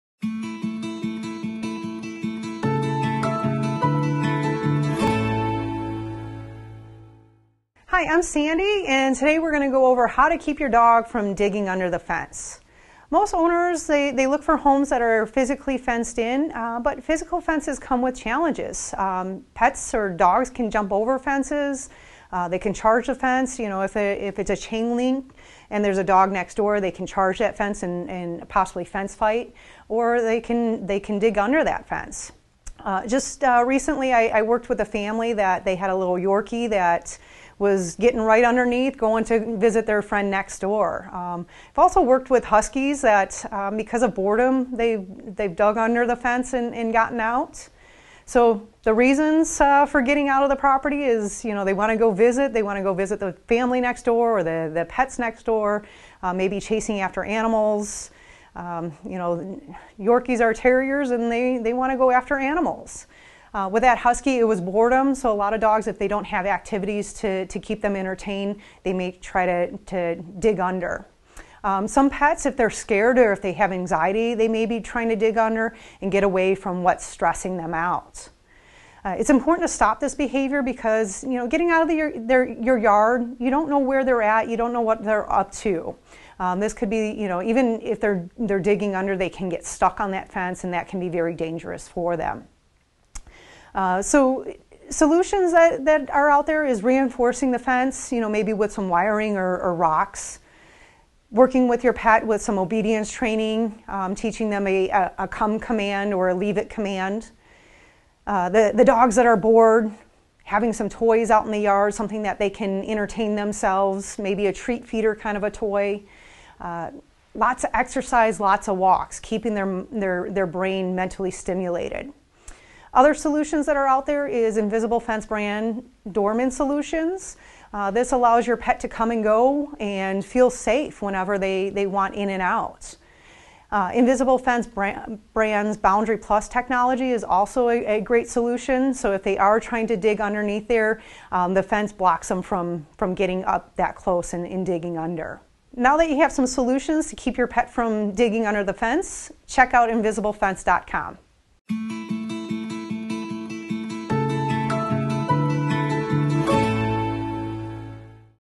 How to Keep Your Dog From Digging Under Your Fence
Find out how you keep your dog from digging under your fence with Invisible Fence® Brand. to learn more about keeping your pets protected.